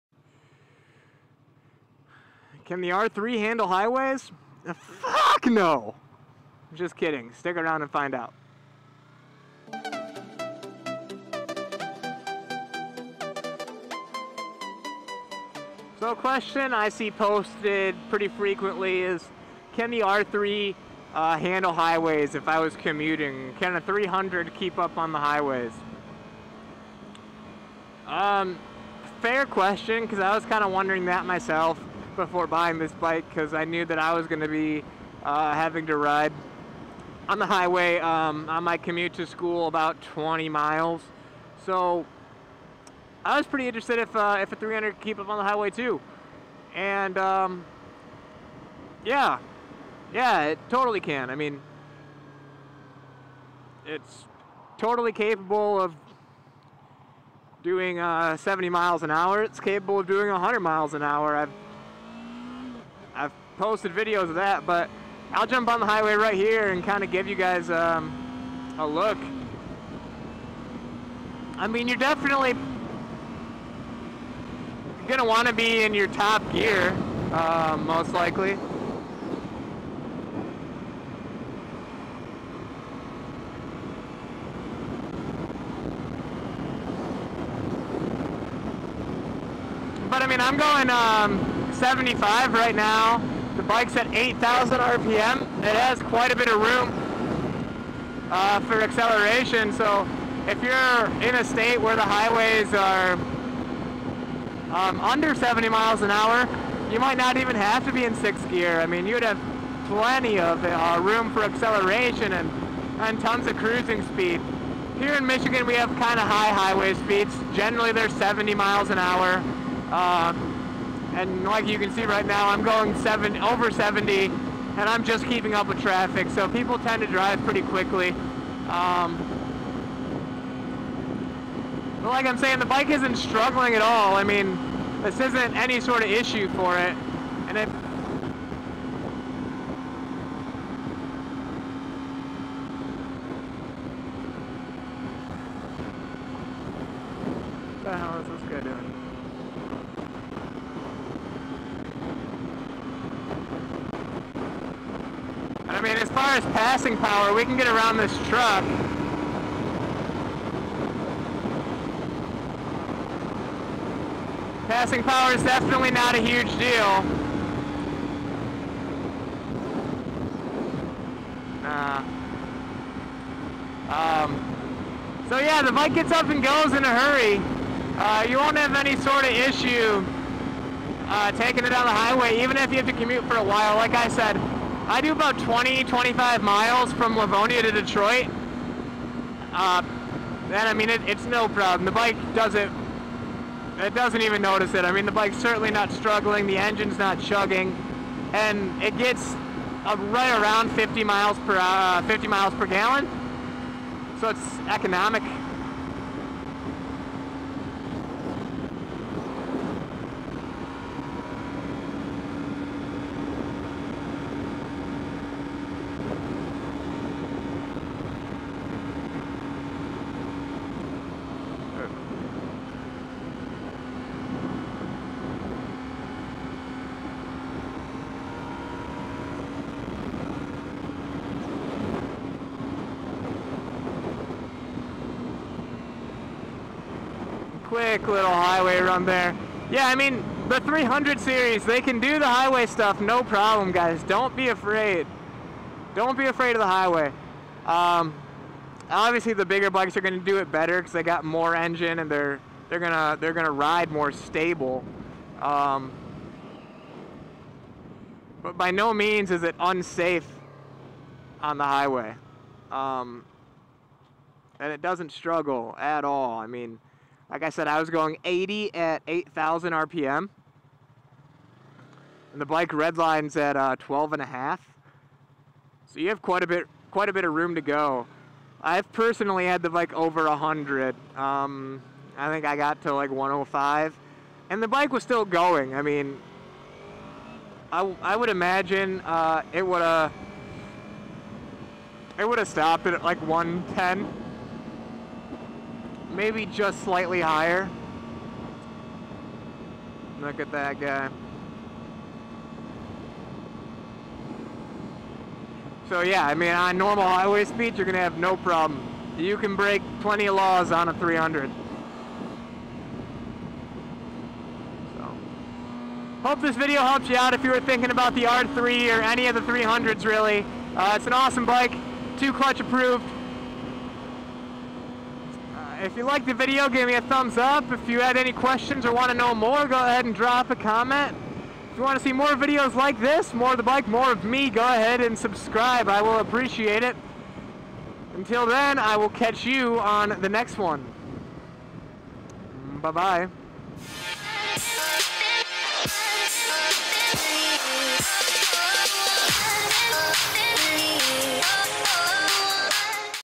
R3 On The Highway + Can It Keep Up?
Can the Yamaha R3 keep up on the highway? Yeah, no problem this motorcycle has all the performance you need to keep up, its amazing on the highway.

Aquul.com- Intro template
Song-